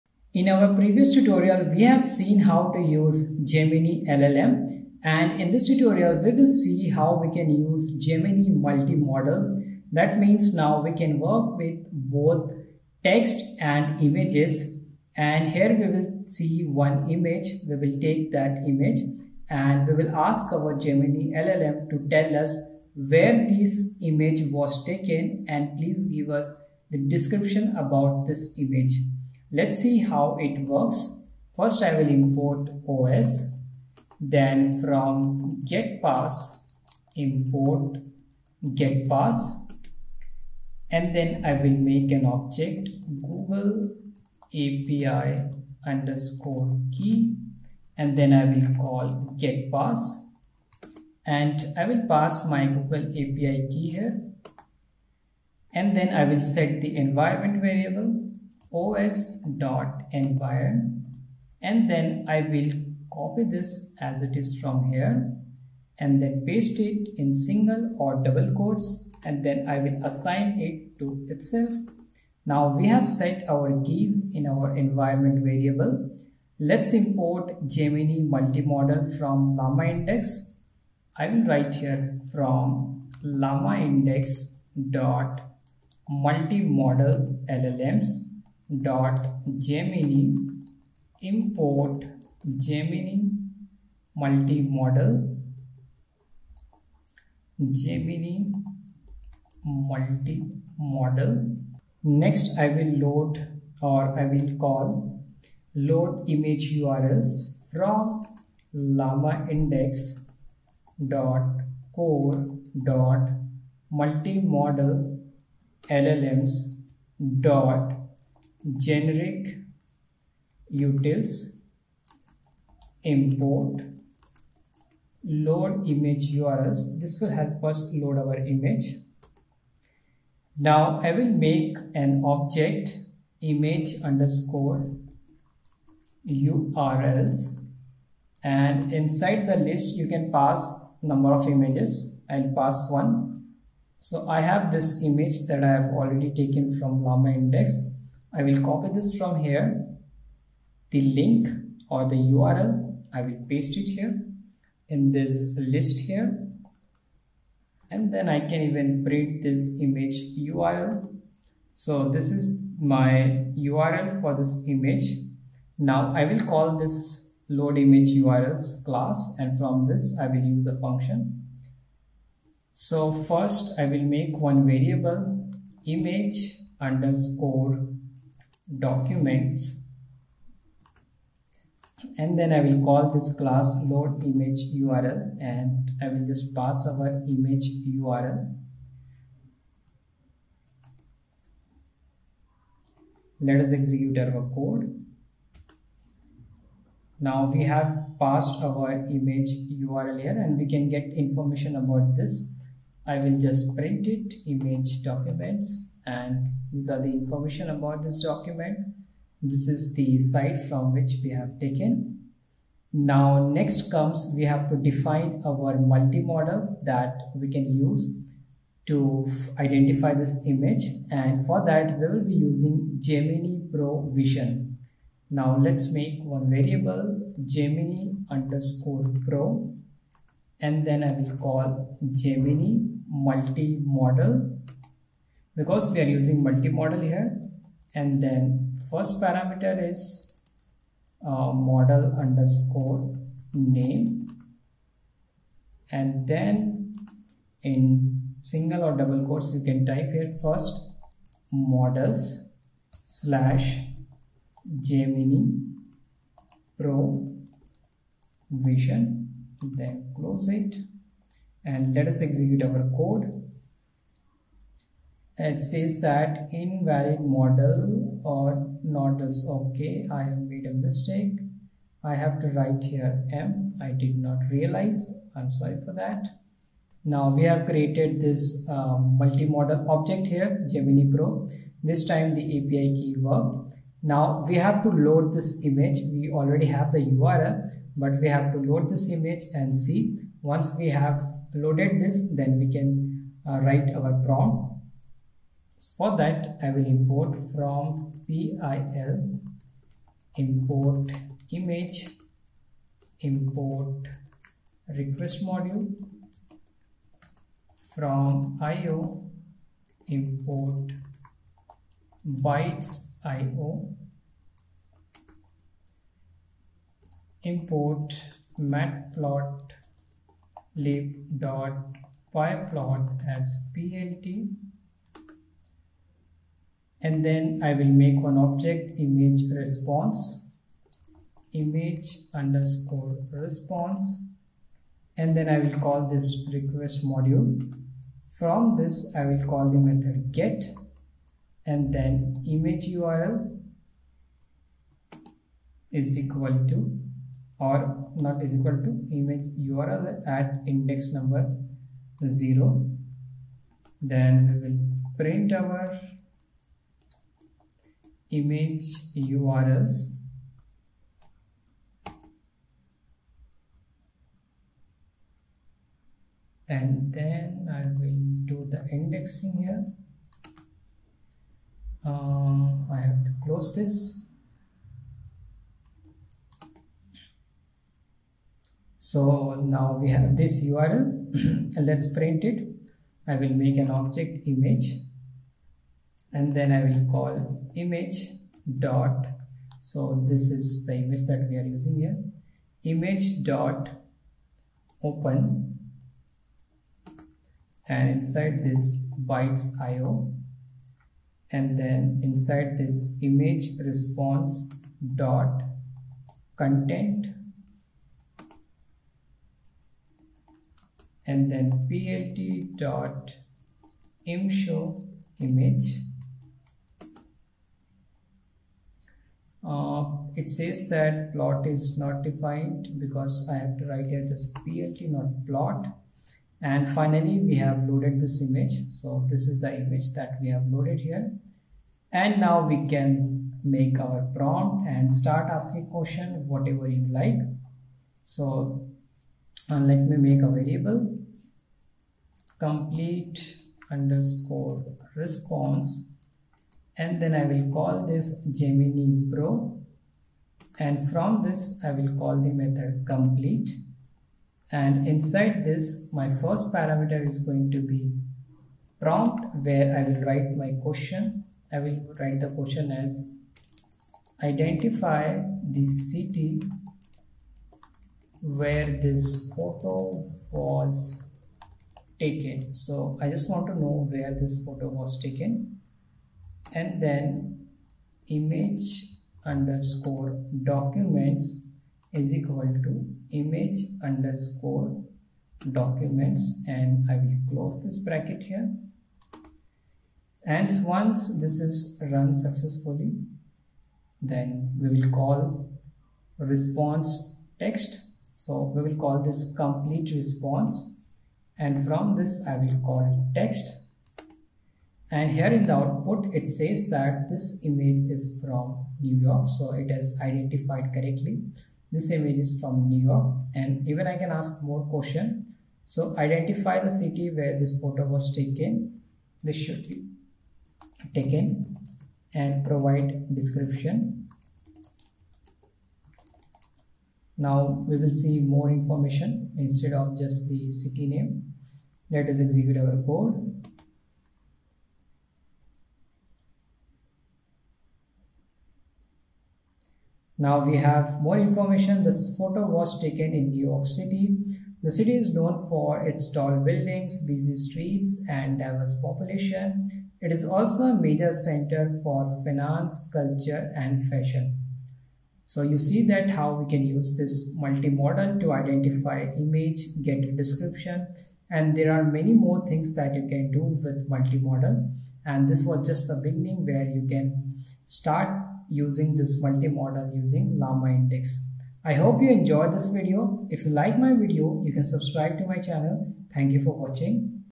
LlamaIndex 17: Google Gemini Pro Vision MultiModal LLM using LlamaIndex | Python | GeminiMultiModal
About this video: In this video, you will learn how to use gemini pro vision multimodal llm